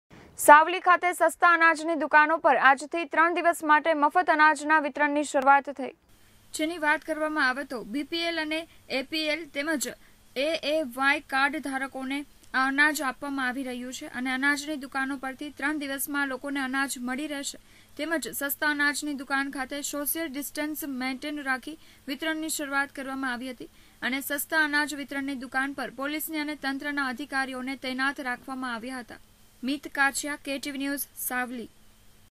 SAVLI: સાવલી ખાતે સસ્તા અનાજની દુકાનો પર આજથી ત્રણ દિવસ માટે મફત અનાજના વિતરણની
SAVLI: સાવલી ખાતે સસ્તા અનાજની દુકાનો પર આજથી ત્રણ દિવસ માટે મફત અનાજના વિતરણની શરૂઆત થઈ.